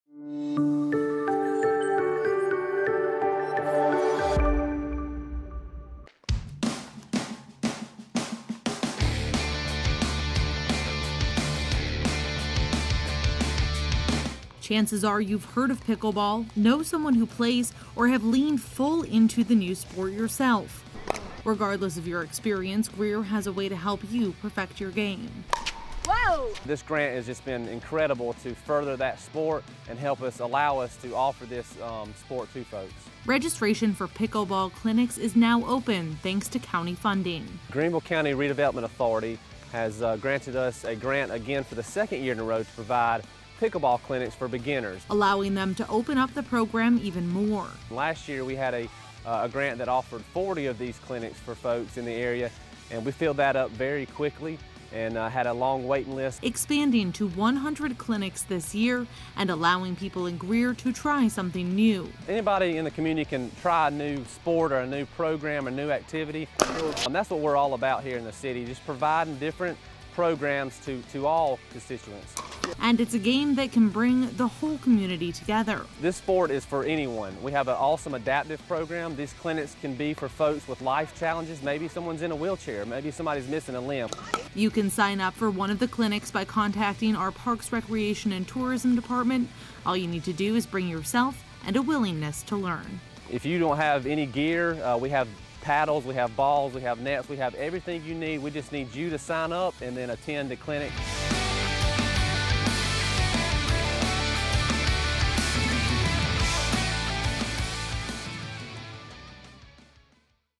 PICKLEBALL CLINIC GRANTS
Have you tried pickleball yet? Now is your chance! Greer Recreation has received a grant to offer 100 beginner clinics this year.